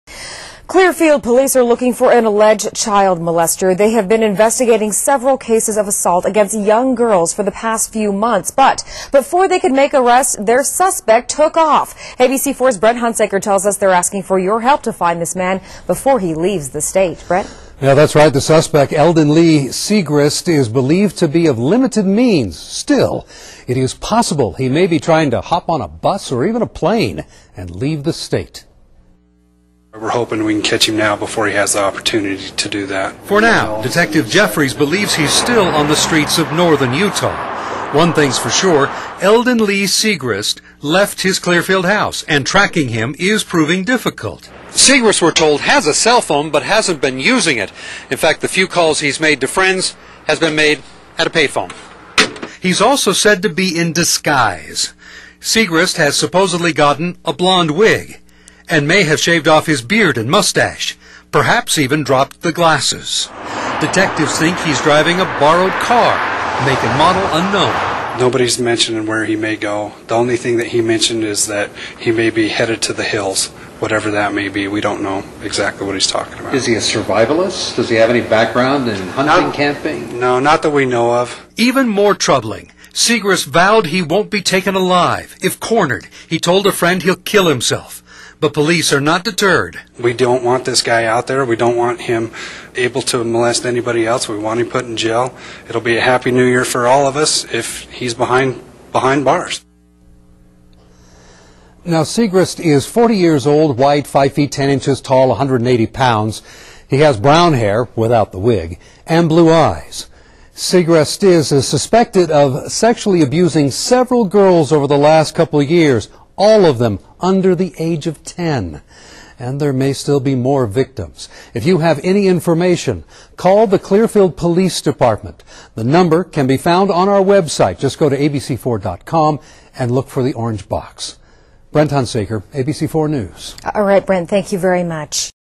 Utah~Manhunt For Alleged Child Molester Eldon Seagrist
Police need you help in locating Eldon Seagrist and getting him behind bars. If you know where this man is please call your area police or 911